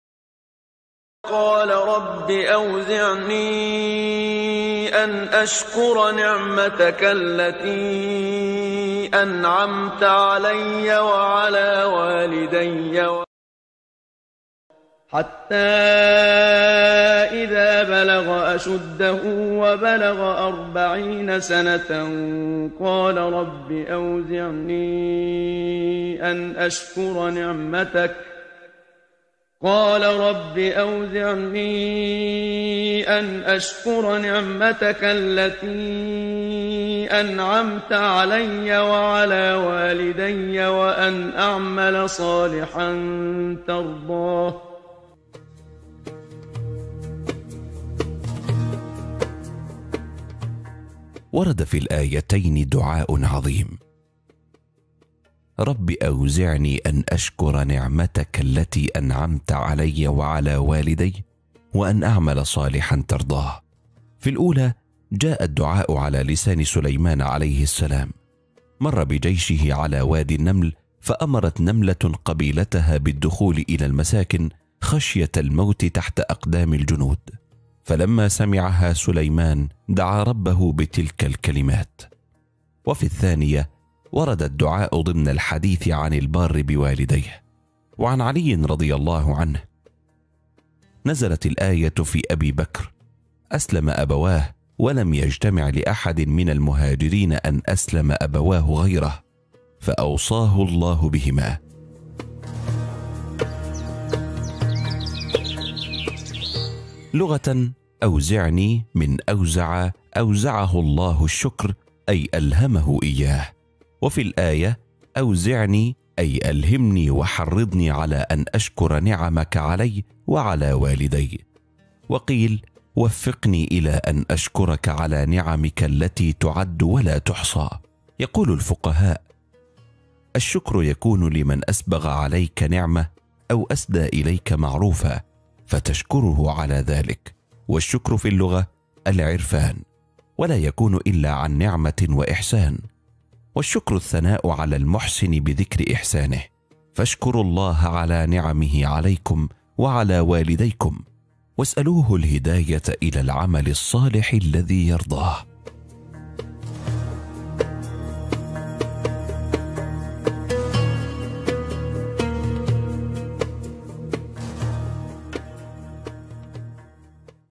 "رب أوزعني أن أشكر نعمتك"
(رَبِّ أَوْزِعْنِي أَنْ أَشْكُرَ نِعْمَتَكَ).. دعاء عظيم ورد في سورتي النمل والأحقاف، فما سر نزوله؟ وماذا يعني؟